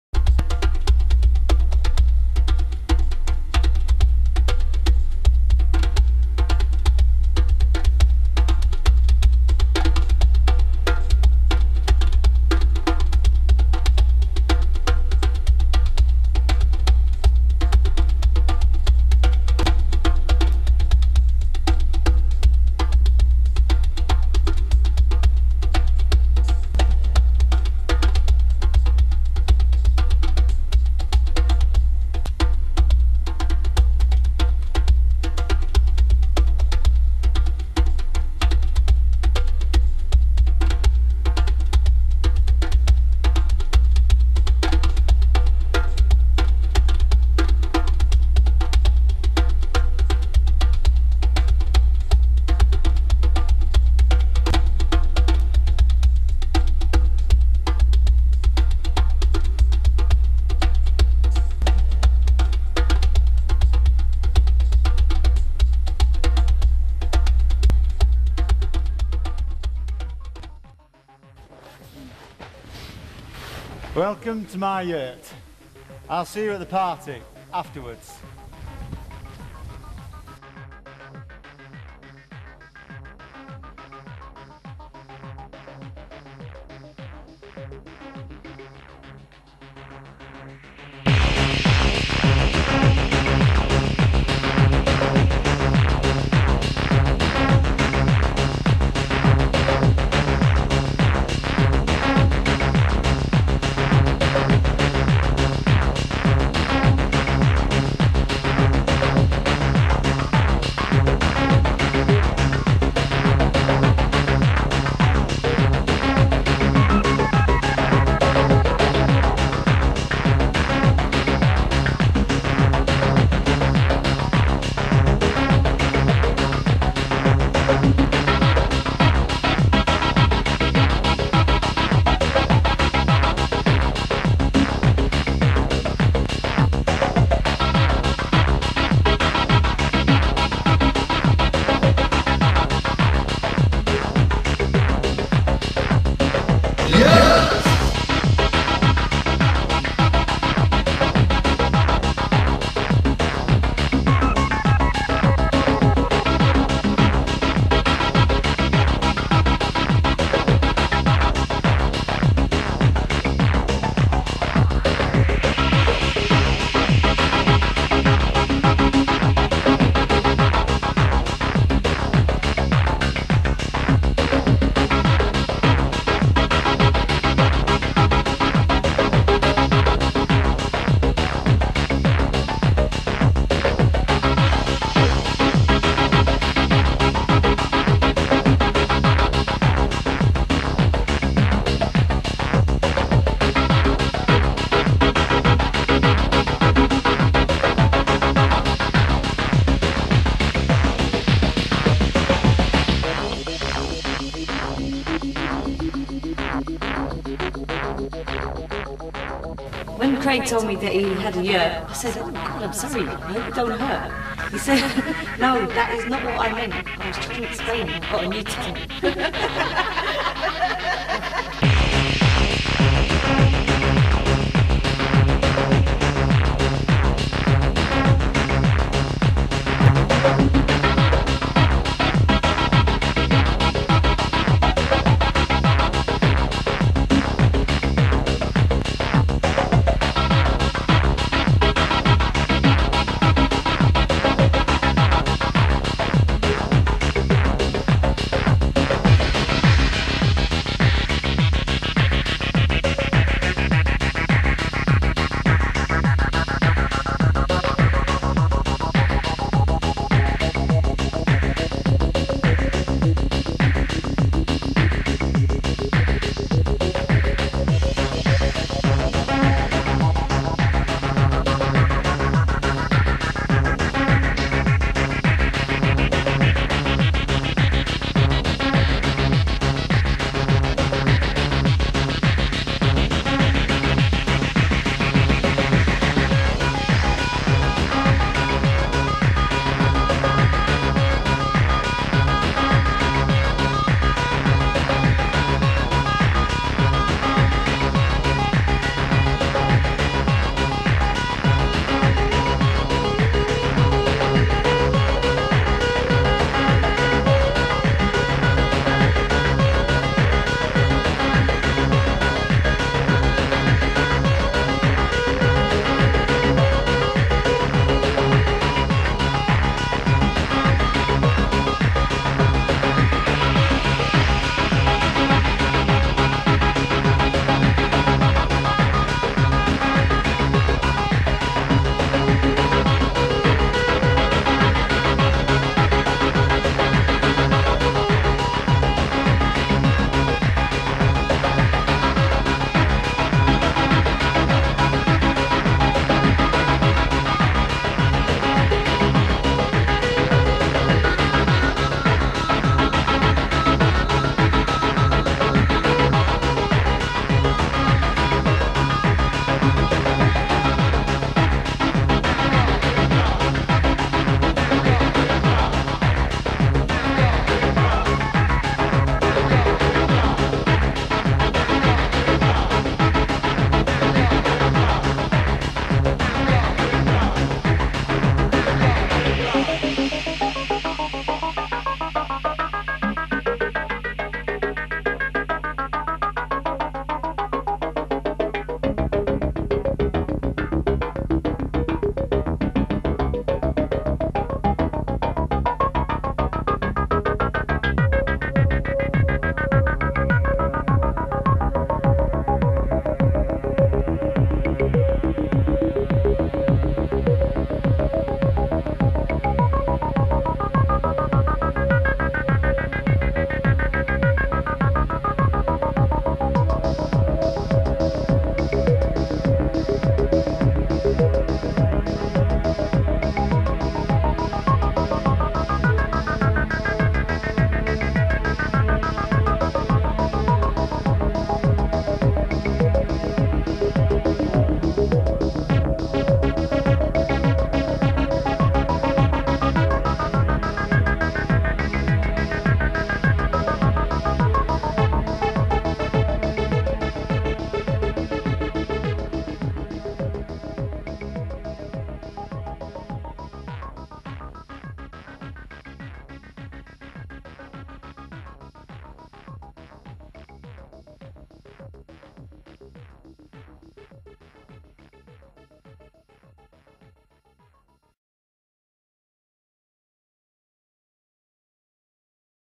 long yurt minus intro
This is the film made in shropshire, lancashire and london. Its fairly self explanitary untill you get to the party at the end. Whats tha all about.